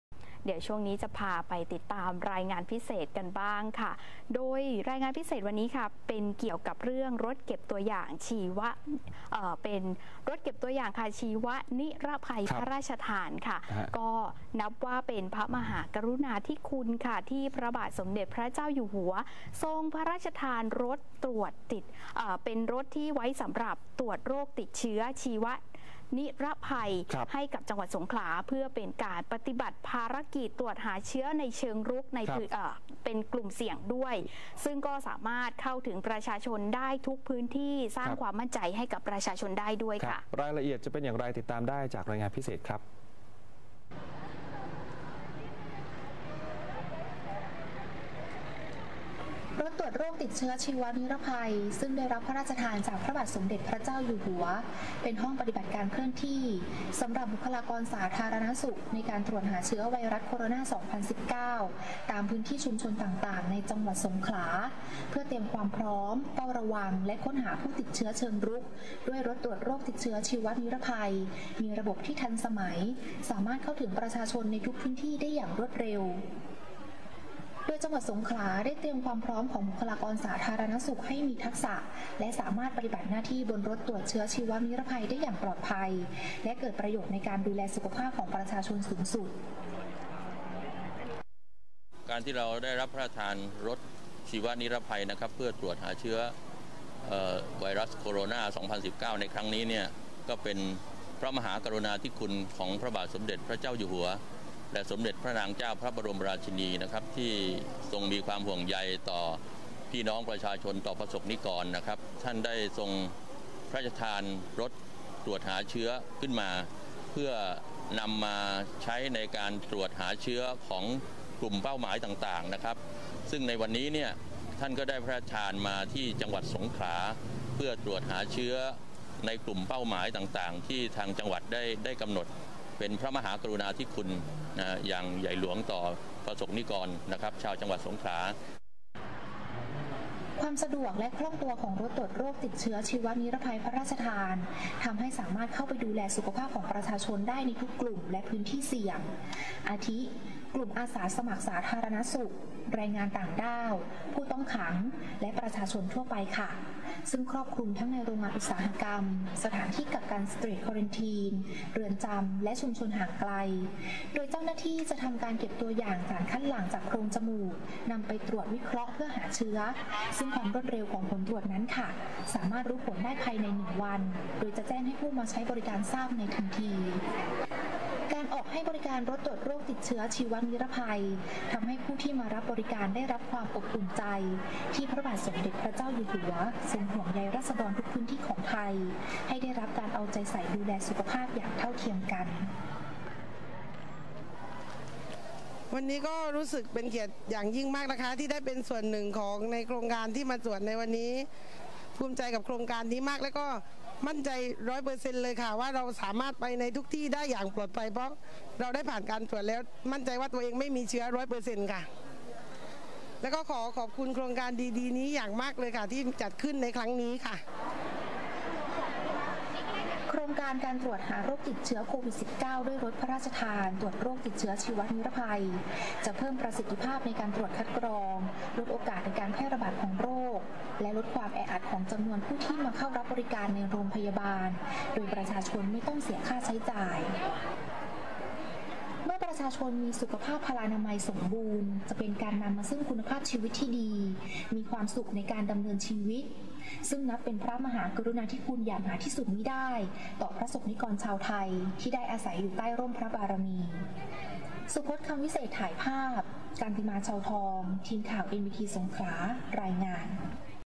รถเก็บตัวอย่างชีวนิรภัยพระราชทาน เข้าถึงทุกพื้นที่รวดเร็ว ปลอดภัย
รถกระบะสีขาวถูกดัดแปลงเป็นห้องปฏิบัติการเคลื่อนที่หรือเรียกว่ารถเก็บตัวอย่างชีวนิรภัยพระราชทาน ในการตรวจหาเชื้อโควิด-19 ให้กับประชาชนกลุ่มเสี่ยงให้เข้าถึงบริการได้อย่างรวดเร็ว นับเป็นพระมหากรุณาธิคุณของพระบาทสมเด็จพระเจ้าอยู่หัว รัชกาลที่ 10 พระราชทาน รถเก็บตัวอย่างชีวนิรภัยพระราชทาน 13 คัน

การลงพื้นที่ตรวจหาเชื้อโควิด-19 แต่ละครั้ง รถเก็บตัวอย่างจะลงไปในพื้นที่เสี่ยงหรือพื้นที่ที่พบผู้ป่วยโรคปอดอักเสบ พร้อมกับแพทย์และเจ้าหน้าที่ เพื่อเก็บตัวอย่างสารคัดหลั่ง โดยเป็นการค้นหาผู้ป่วยโควิด-19 ในเชิงรุก

ไม่เพียงแต่ประสิทธิภาพและความปลอดภัยในการลงพื้นที่ปฏิบัติงาน แต่รถคันนี้ยังเคลื่อนที่ไว ลงตรวจหาเชื้อให้กลุ่มเสี่ยงและผู้ที่ยากไร้ให้ง่ายต่อการเข้าถึงบริการ ซึ่งการตรวจแต่ละคนจะใช้เวลาไม่ถึง 10 วินาที โดย 1 คัน สามารถตรวจได้ถึง 800-1000 ตัวอย่าง ใช้เวลาประมาณ 6 ชั่วโมง ต่อวัน

บทเรียนจากการตรวจพบผู้ติดเชื้อโควิด-19 ที่เดินทางมาจากต่างประเทศ เข้าพักจังหวัดระยอง และกรุงเทพมหานคร ทำให้ทุกภาคส่วนต้องหันมาให้ความสำคัญเรื่องการกักตัว ยกการ์ดไว้ไม่ให้ตก จนกว่าสถานการณ์โควิด-19 ทั่วโลก จะดีขึ้น